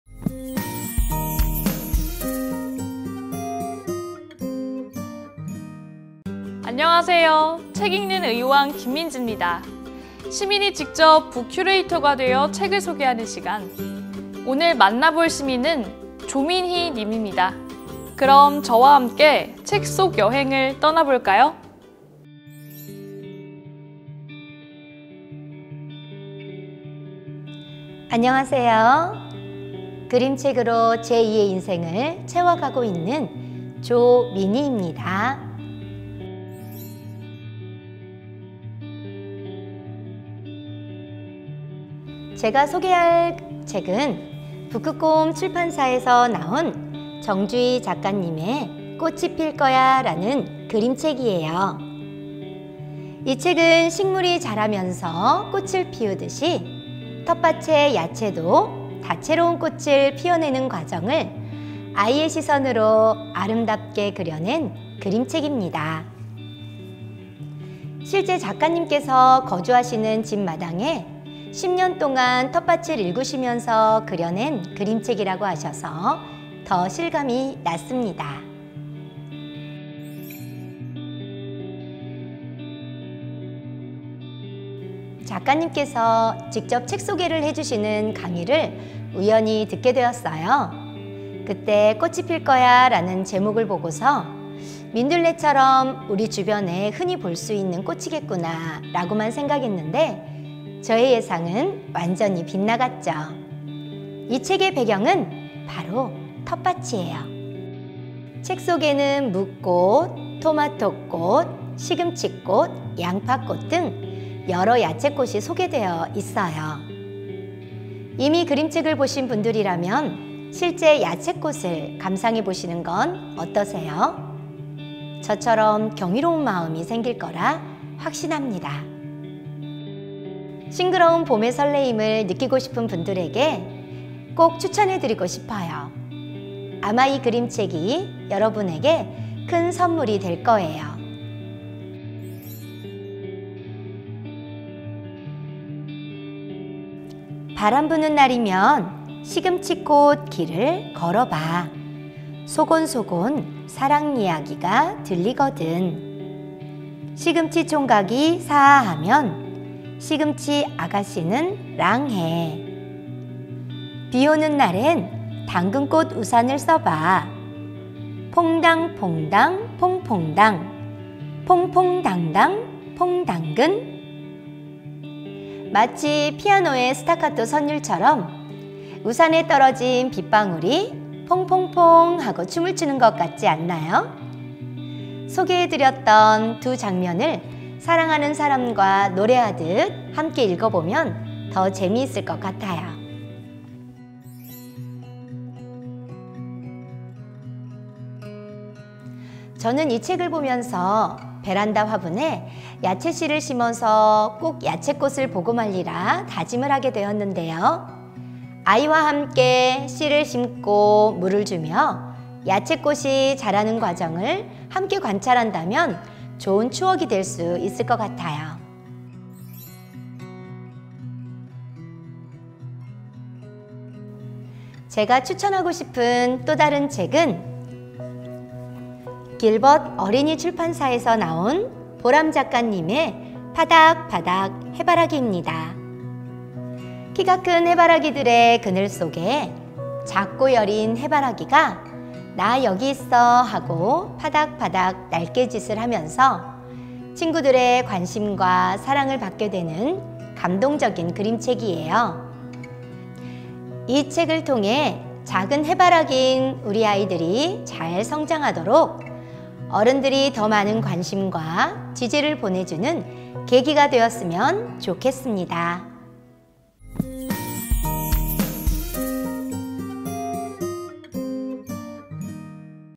📚2023 책 읽는 의왕📖ㅣ시민 북큐레이션 1화
시민이 직접 📙책을 소개하고 들려주는 시민 북큐레이션
그 첫번 째 시간은 ✨조민희님✨입니다
우리에게 어떤 책을 선물할지 다 함께 볼까요?👏😍